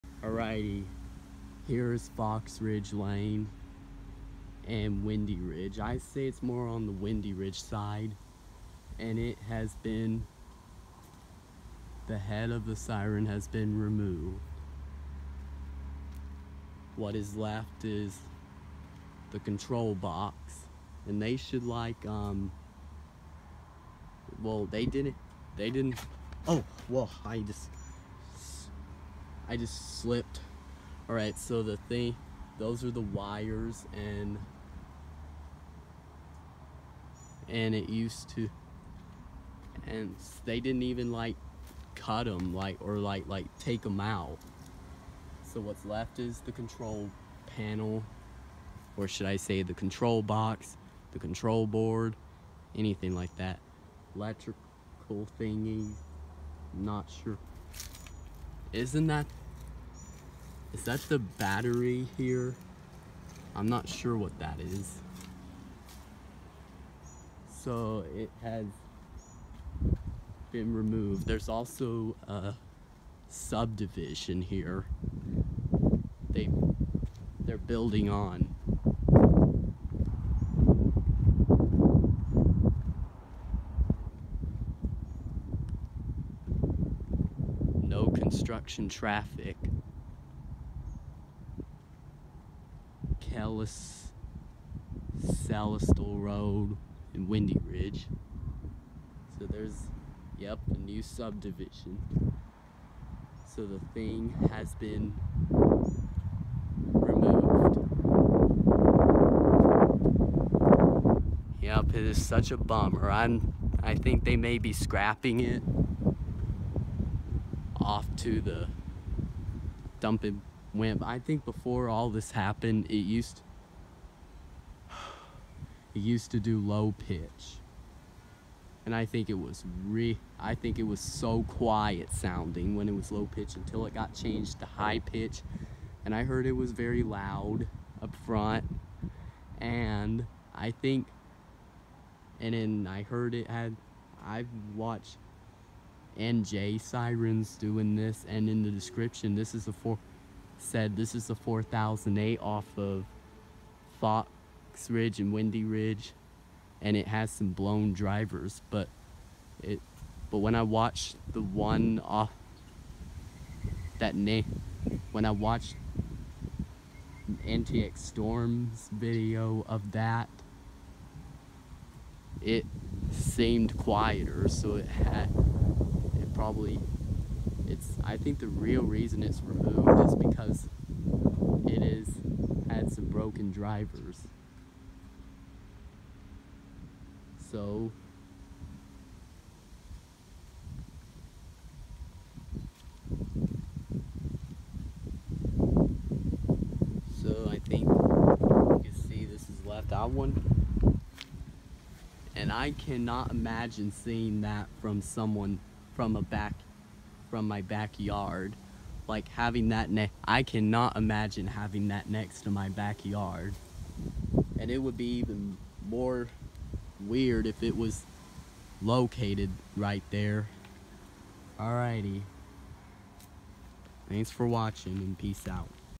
Whelen wps 4008 removed :(
I went there last month back in June but didn’t have time to film it removed before this happened it has had a low pitched bed possible protection and cuts out while the others kept going can y’all tell me in the comments if it had a low battery because it cut out while the others kept going and when it got changed to high pitch it was alot louder until the drivers wore out I hope they just have it down to fix the drivers or put in new drivers tell what you think is it being replaced or getting new drivers all that I want to know

Camera: iPhone 6’s
Location: fux ridge ln on and windy ridge rd
Person met up: no one else